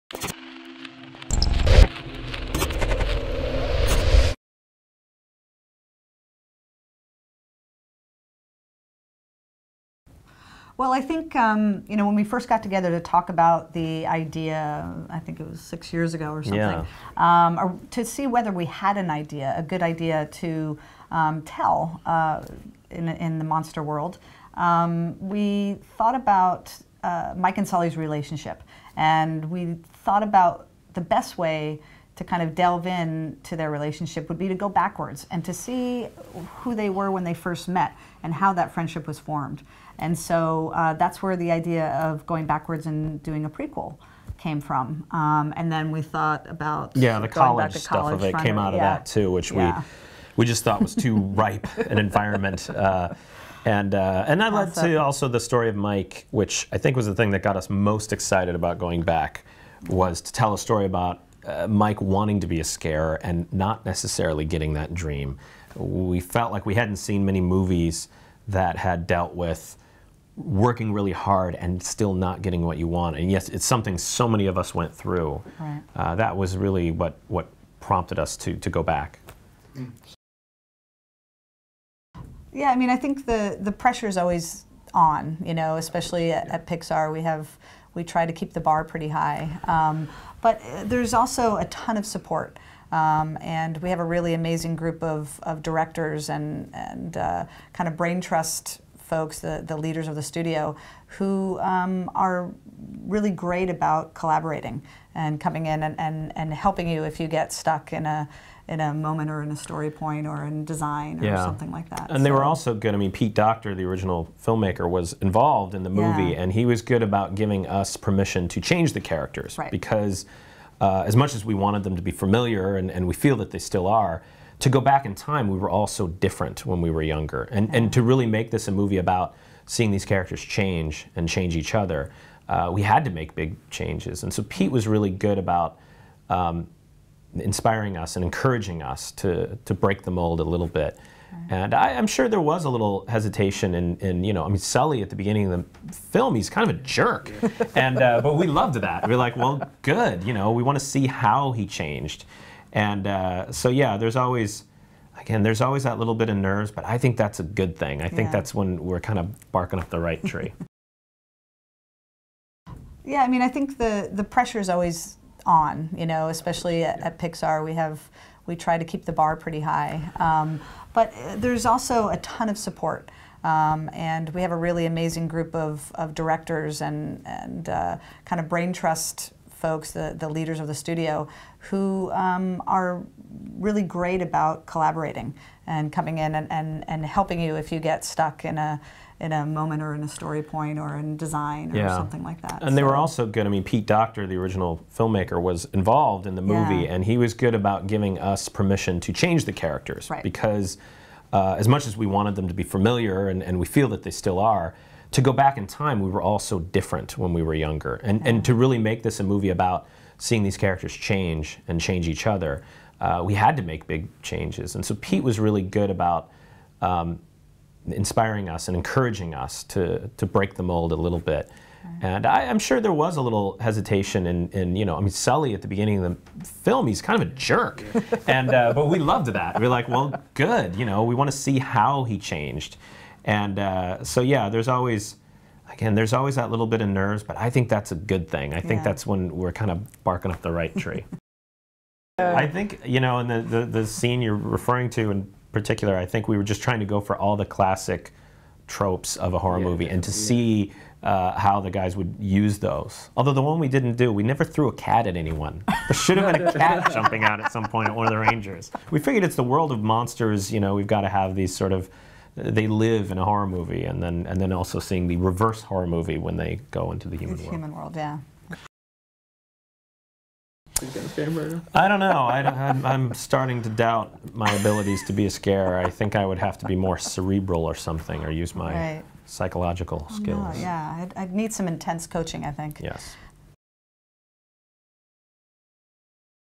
Monsters University Dan Scanlon Kori Rae Interview
We sit down and talk with director Dan Scanlon and Pixar producer Kori Rae to discuss Monsters University.